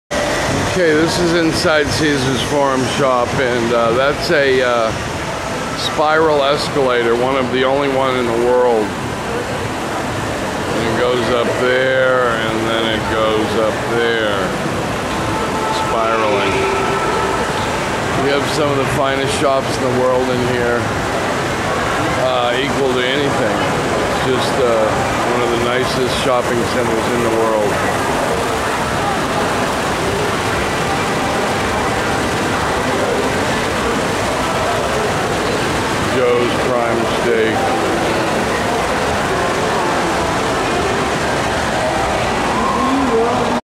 Visit Caesars Forum Shops Las Vegas Nevada
Todd Johnson presents One of the only spiral escalators in the world at the entrance to Forum Shops.